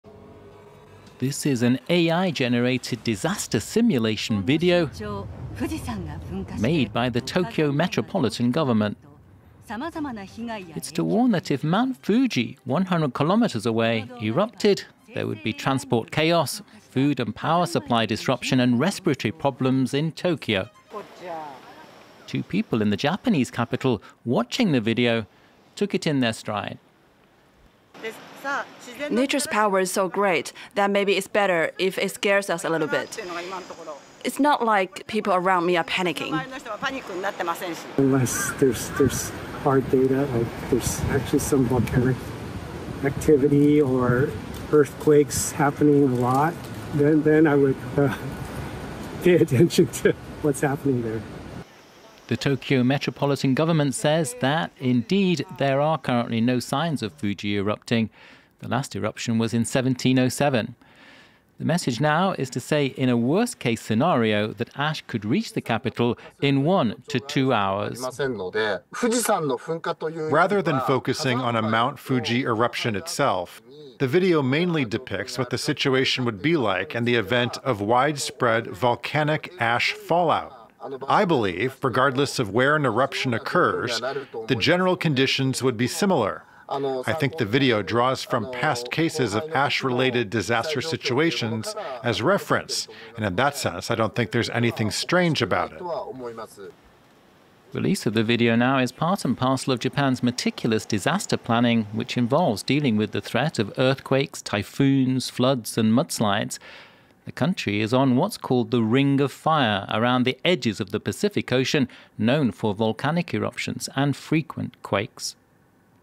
An AI simulation of a Mount Fuji eruption is being used to prepare Tokyo for the worst • FRANCE 24
Mount Fuji hasn’t erupted since 1707. But for Volcanic Disaster Preparedness Day, Japanese officials have released computer- and AI-generated videos showing a simulation of a potential violent eruption of the active volcano.